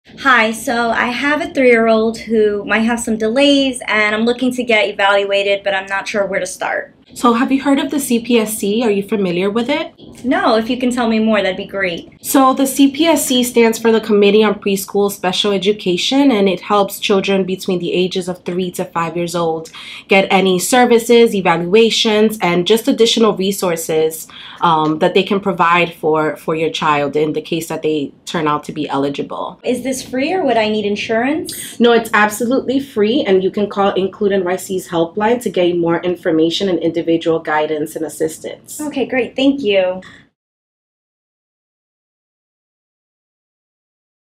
Committee on Preschool Special Education (CPSE)
The Committee on Preschool Special Education (CPSE) manages the process for special education services for children ages 3-5. Learn more about how to get started!